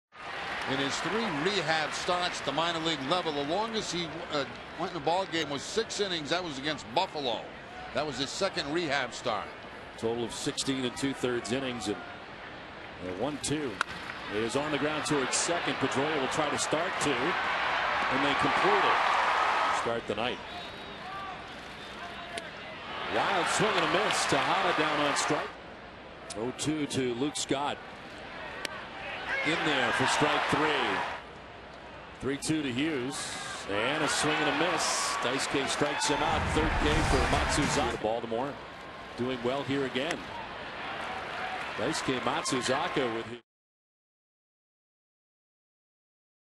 2010/05/01 Dice-K's 2010 debut
BOS@BAL: Matsuzaka makes his first start of 2010